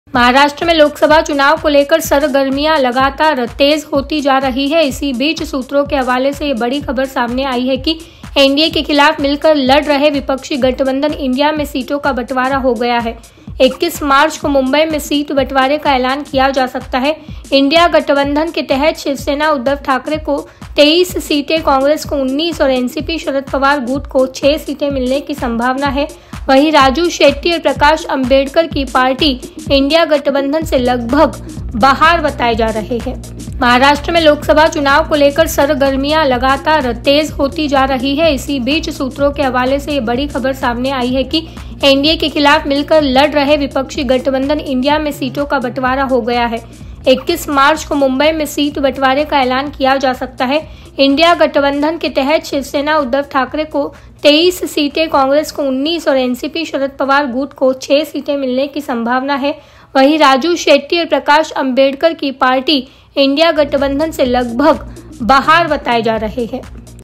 I.N.D.I.A Alliance सीटों के बंटवारे को लेकर जल्द ही करेगा ऐलान
महाराष्ट्र में लोकसभा चुनाव को लेकर सरगर्मियां लगातार तेज होती जा रही हैं. इसी बीच सूत्रों के हवाले से बड़ी खबर सामने आई है कि एनडीए के खिलाफ मिलकर लड़ रहे विपक्षी गठबंधन इंडिया में सीटों का बंटवारा हो गया है. 21 मार्च को मुंबई में सीट बंटवारे का एलान किया जा सकता है. इंडिया गठंबधन के तहत शिव सेना उद्धव ठाकरे को 23 सीटें, कांग्रेस को 19 और एनसीपी शरद पवार गुट को 6 सीटें मिलने की संभावना हैं. वहीं राजू शेट्टी और प्रकाश अंबेडकर की पार्टी इंडिया गठबंधन से लगभग बाहर बताए जा रहे हैं.